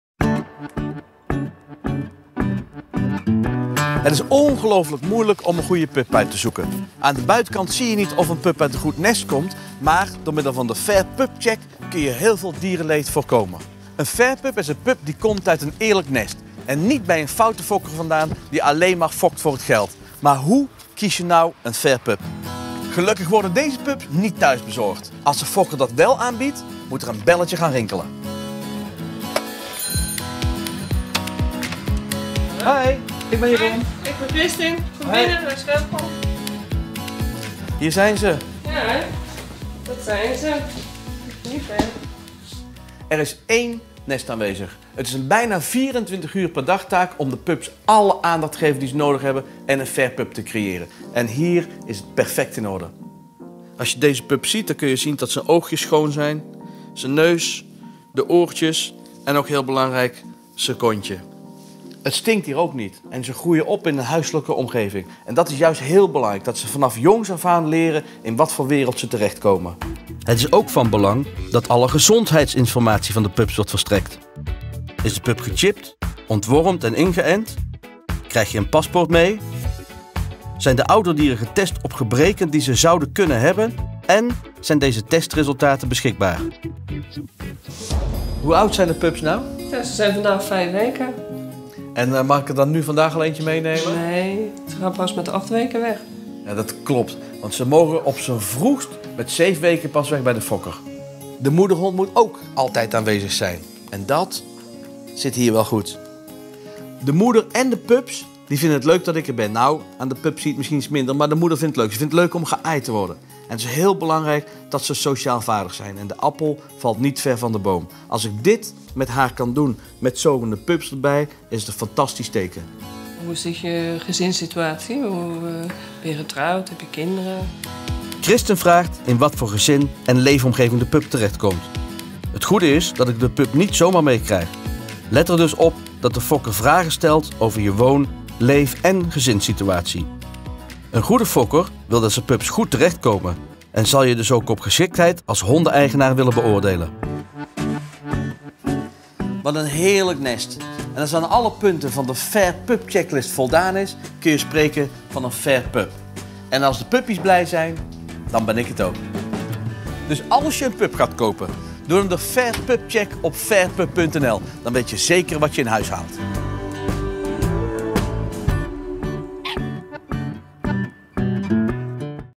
FAIRPUP check; campagne van de Koninklijke Hondenbescherming
Voor de Koninklijke Hondenbescherming heb ik meegewerkt aan hun campagne in het najaar van 2014. De campagne is erop gericht mensen inzicht te verschaffen in het aanschaffen van een goede (een FAIR)pup.

In dit filmpje is de FAIRPUP CHECK te zien...